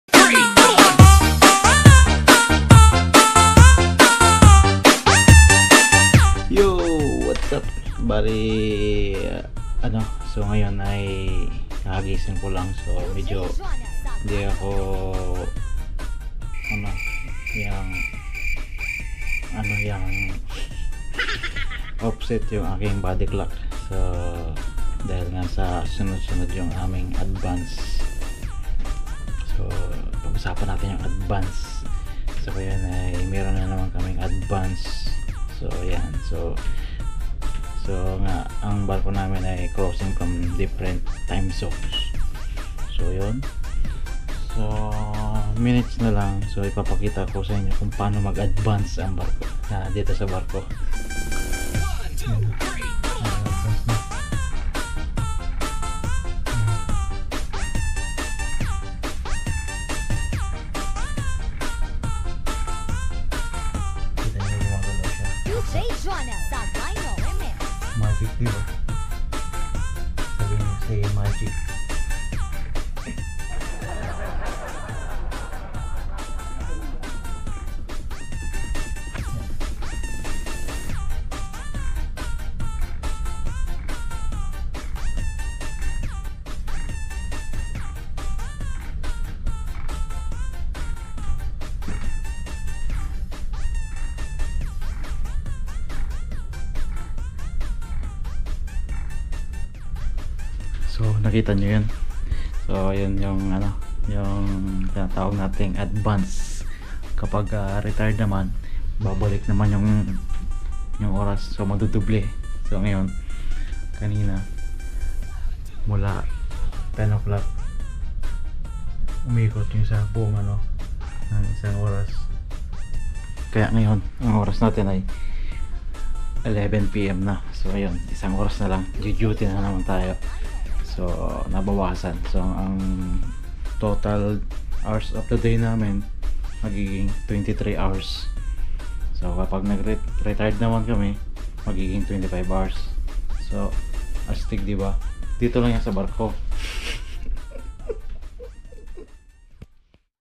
Time Adjustment sa Barko l shipsclock l
Time Adjustment sa Barko paano nga ba nangyayari?

Sa video na ito makikita na nag aadvance ang aming Relo/orasan

Advance ang term kapag mag plus ng oras
Retard ang term kapag mag minus ng oras